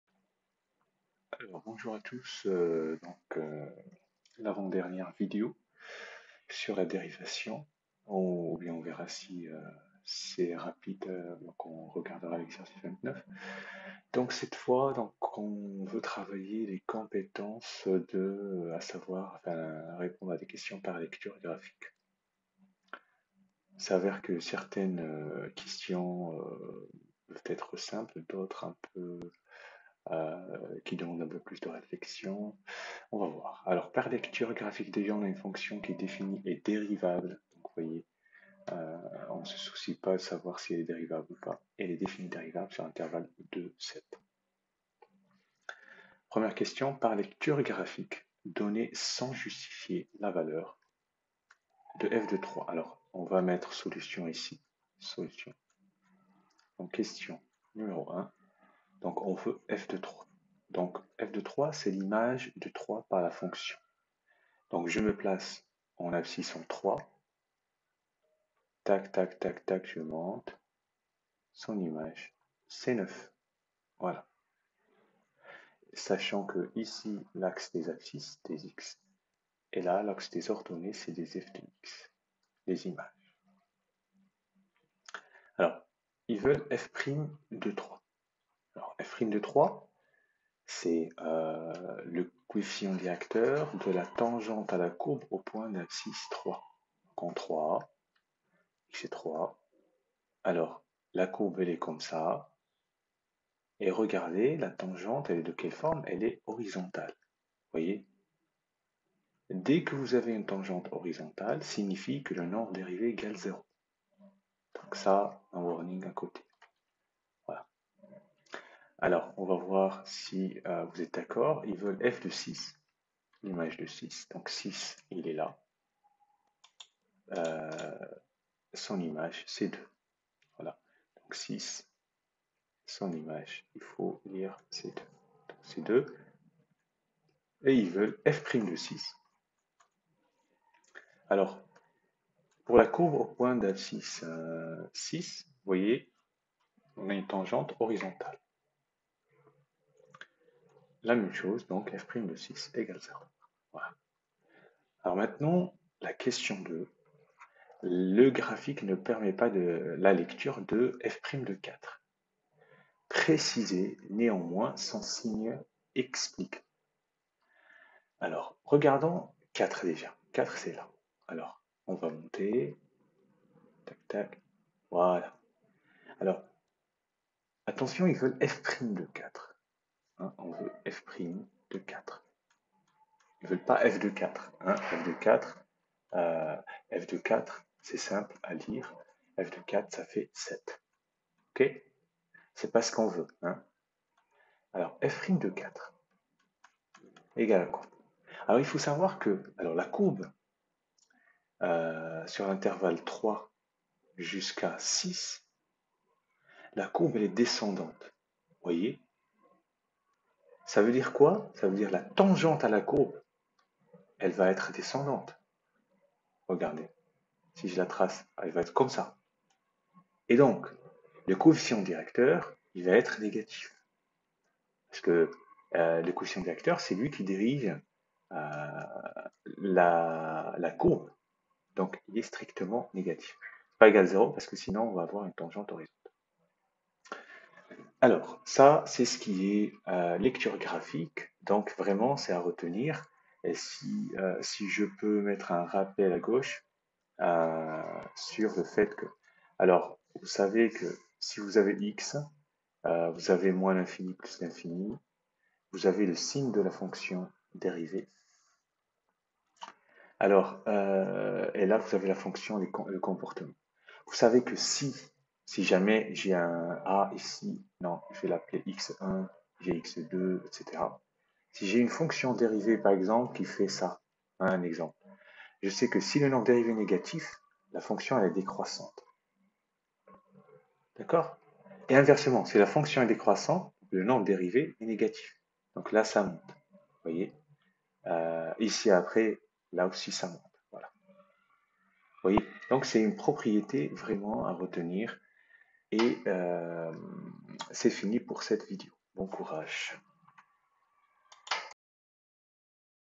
lecture graphique du nombre dérivé
Dans cette vidéo, vous avez la correction de l'exercice 28 sur la lecture graphique du nombre dérivé en un point de la courbe, l'image d'un nombre par une fonction ainsi que le signe de la dérivée à partir du graphique.
Bon courage.